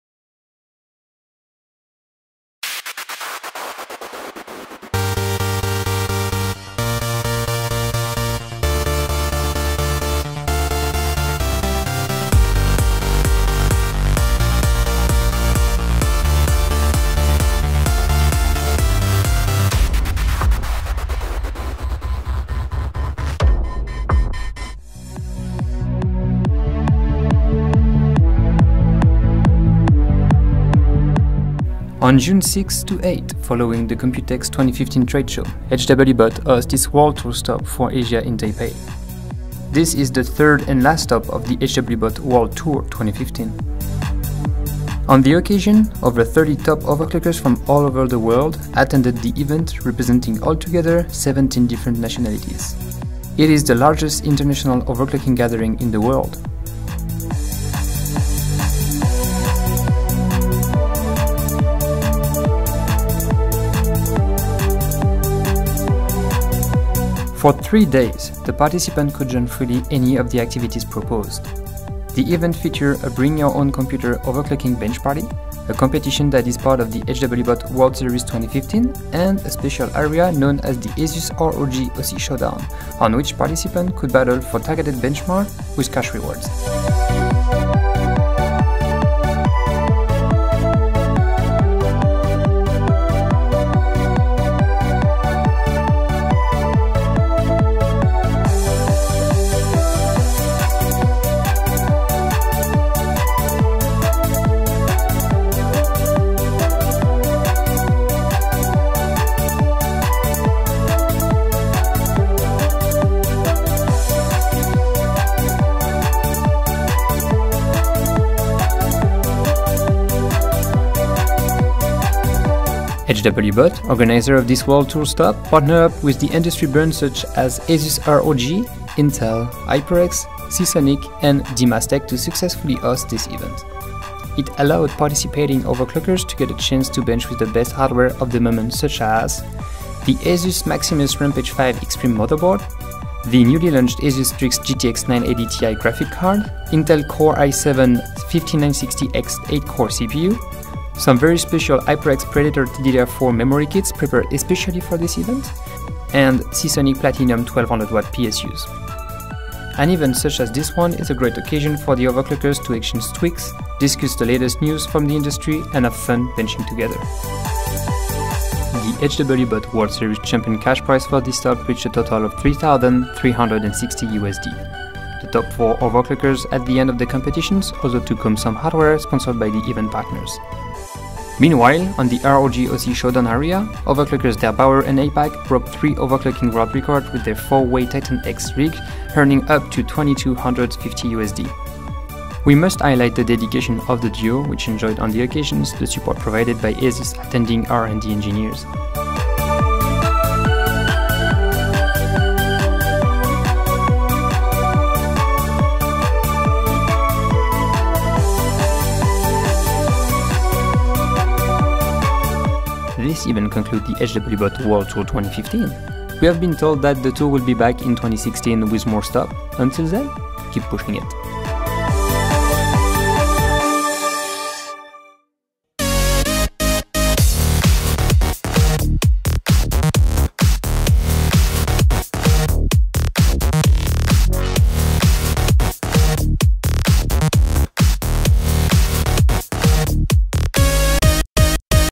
Aftermovie - HWBOT World Tour 2015 Asia - ROG OC Showdown
This is the official aftermovie from the HWBOT World Tour 2015 - ROG OC Showdown Asia stop held in Taipei Taiwan. This is the world's largest Overclocking gathering.

- ASUS ROG Matrix 290X Graphics Card: Brent C.
- ASUS ROG Maximus VII Gene Motherboard: Charles D.
- HyperX Predator DDR4 4x4GB Memory kit: Kelly M.
- Seasonic Snow Silent Platinum Power Supply: Thomas S.